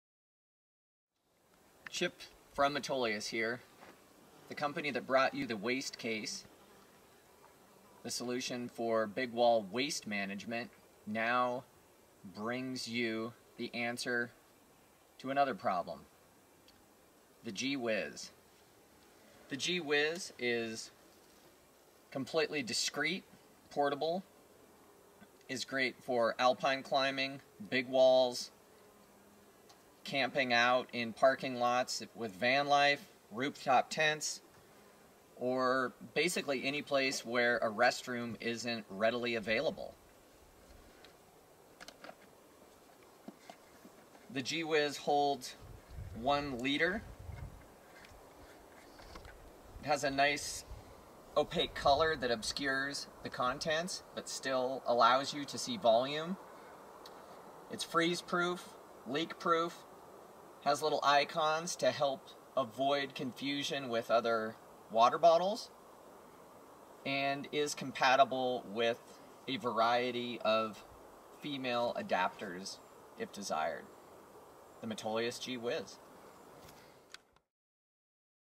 G-Whiz
The G-Whiz is a portable, discreet solution for relief at sensitive areas, big walls, van life, etc. Leak-proof, freezable, lightweight, and portable. Obscured color allows you to see volume, but not contents.

• Roll storage with tether in collar
• Multi-gender compatibility that accepts female specific adapters if preferred
• Volume: 33.8 oz. (1 L)
• Weight: 5.2 oz. (147 g)
• Color: Green